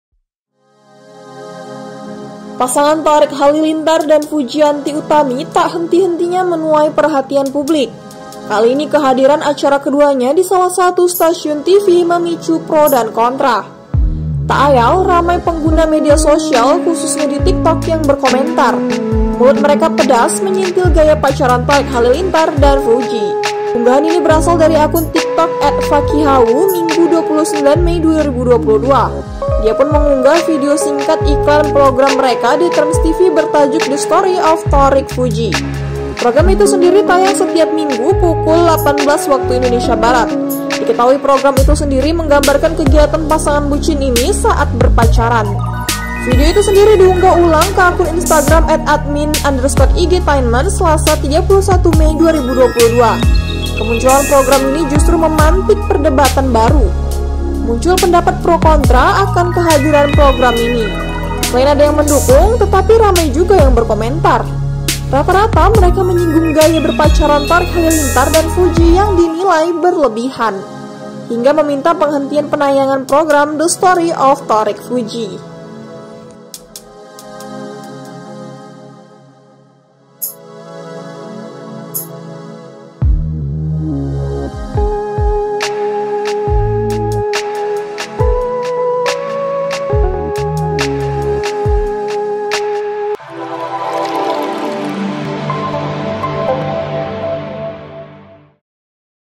KEBUCINAN Thariq dan Fuji Tuai Kritikan Netizen, Minta Program Tv The Story of Thariq Fuji Dihapus
TRIBUN-MEDAN.COM - Pasangan Thariq Halilintar dan Fujianti Utami tak henti-hentinya menuai perhatian publik.

Kali ini kehadiran acara keduanya di salah satu stasiun TV memicu pro-kontra.

Mulut mereka pedas menyentil gaya pacaran Thariq Halilintar dan Fuji.

Unggahan ini berasal dari akun TikTok @fakihawu, Minggu (29/5/2022).

Dia pun mengunggah video singkat iklan program mereka di TRANS TV bertajuk 'The Story of Thariq Fuji'.

Program itu sendiri tayang setiap Minggu pukul 18.00 WIB.

Diketahui, program itu sendiri menggambarkan kegiatan pasangan bucin ini saat berpacaran.

Video itu sendiri diunggah ulang ke akun Instagram @admin_igtainment , Selasa (31/5/2022).

Kemunculan program ini justru memantik perdebatan baru.

Muncul pendapat pro-kontra akan kehadiran program ini.

Selain ada yang mendukung, tetapi ramai juga yang berkomentar.

Rata-rata mereka menyinggung gaya berpacaran Thariq Halilintara dan Fuji yang dinilai berlebihan.

Hingga meminta penghentian penayanangan program 'The Story of Thariq Fuji'.